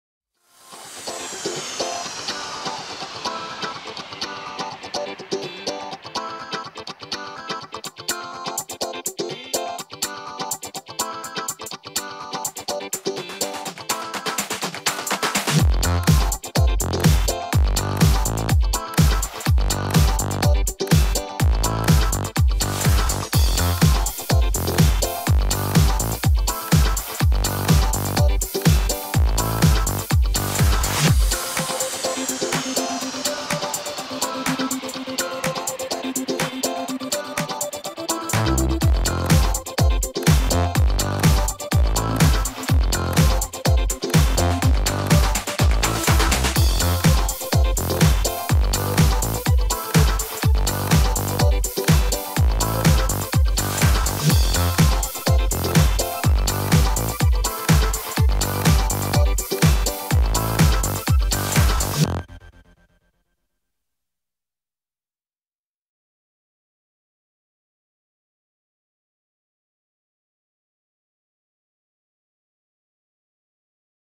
Torre Eiffel spettacolo di luci
Torre Eiffel spettacolo di luci, bellissima la tour eiffel di parigi, la sera si illumina e brilla